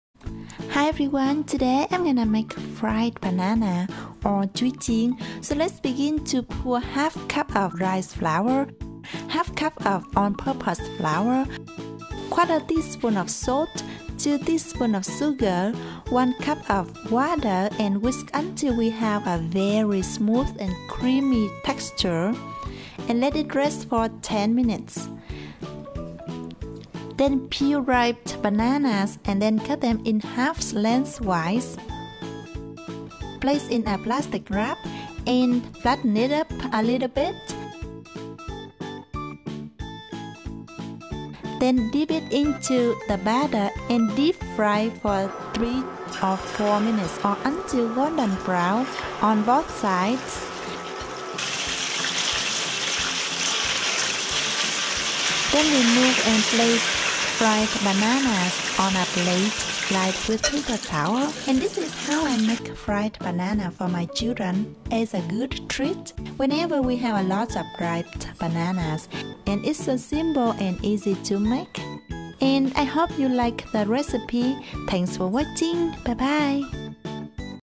Street Food Fried Bananas Recipe (Chuối Chiên) - NPFamily Recipes
For this week recipe, we're making one of the popular street food dessert in Vietnam, Crispy Fried Bananas (Chuoi Chien). This desset is very popular in Vietnam as they're really cheap for school students. You can find them pretty much at any corner and every shool from elementary to high school. The main ingredient is, of course, ripe bananas. Use only ripe bananas as they are at their sweetest point. We are using Siamese banana, (chuối xiêm/chuối sứ) for this recipe (available at Asian markets). However, you can use any kind of bananas.

10 bananas (ripe)
1/2 cup rice flour
1/2 cup all-purpose flour
1/2 tsp baking powder (optional)
2 drop yellow food coloring (optional)
1/4 tsp salt
2 tsp sugar
1 cup water
vegetable oil